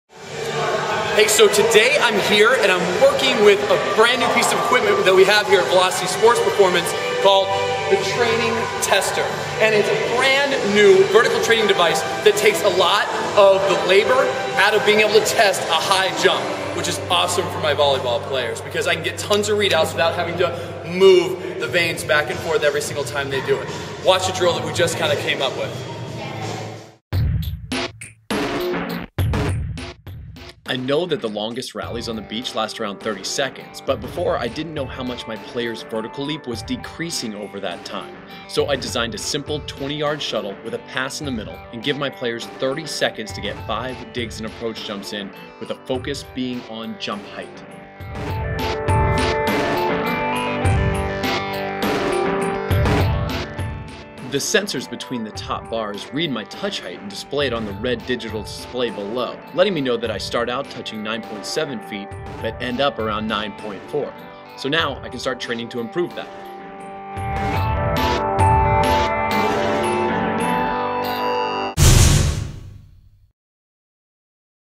Testing Vertical Jump Decline During Long Volleyball Rallies
It was like Christmas this month when Allesandro and Diego came over from Italy to show me their new jump testing and training device, the Training Tester.
Fortunately they've left it with me to play with for a while so here I'm demonstrating how much my vertical jump decreases during a 30 second rally.  Cool new product.